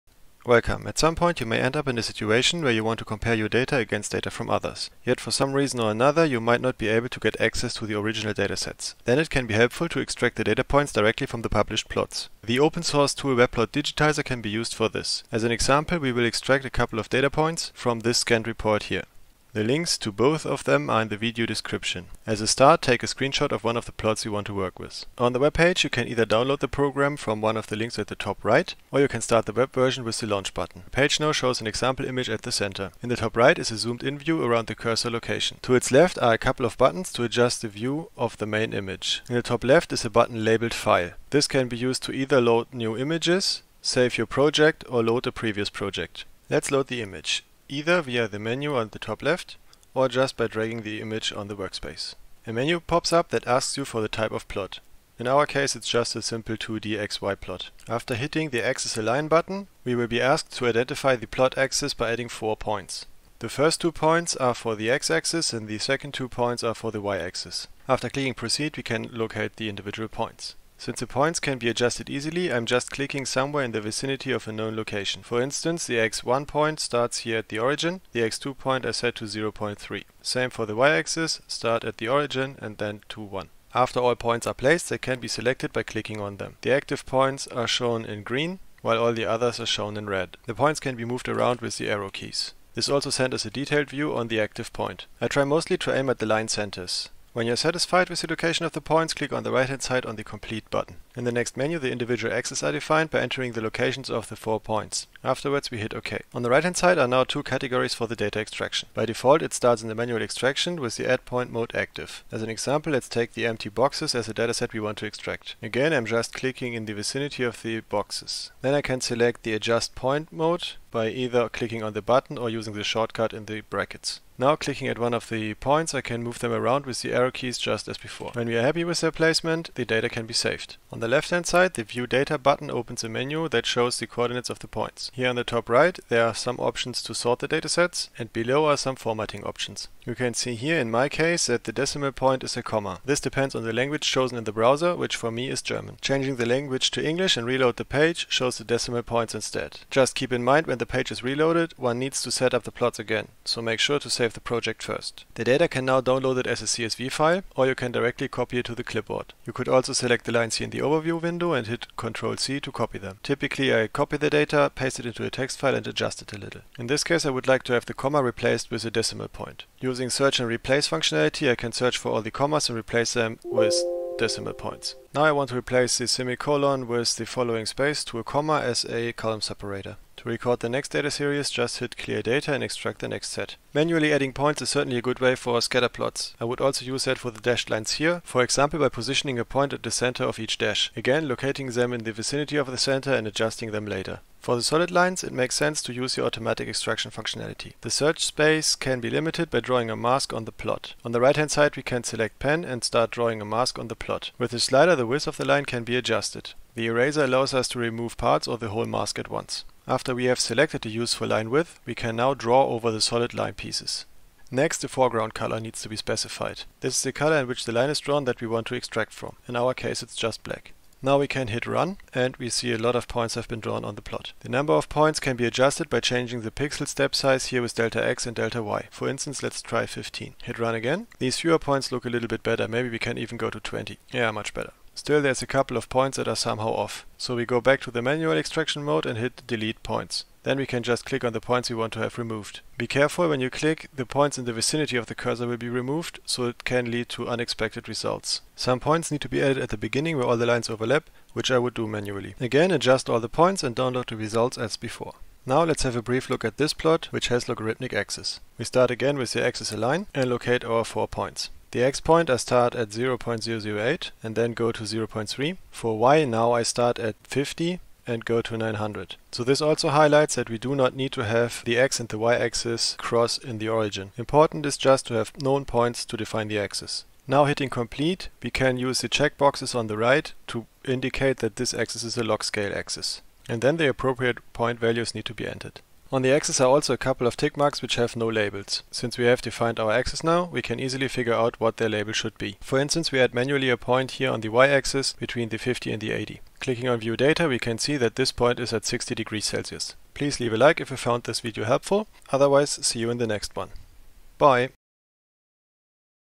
How to Extract Data Points from Plots | WebPlotDigitizer Tutorial
Introduction to the WebPlotdigitizer used to extract data points from images of plots. We focus on 2d plots and cover how to align the axis, automatically extract lines, save the data points and how to use plots with logarithmic axis.
Software like WebPlotdigitizer is specifically useful when to the original data sets are not accessible. Just take a screen shot from the published plot and extract the desired data points.
This should provide a basic overview on how to use the WebPlotdigitizer.

0:00 Load image
0:51 Align axis
2:09 Save data points
3:13 Automatically extract lines
4:14 Logarithmic axis